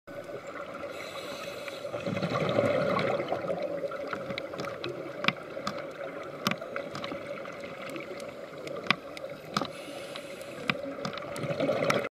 Nudibranch glossodoris hikuerensis
Big nudibranct glossodoris hikuerensis while a dive at St. John's area in the red sea, Egypt.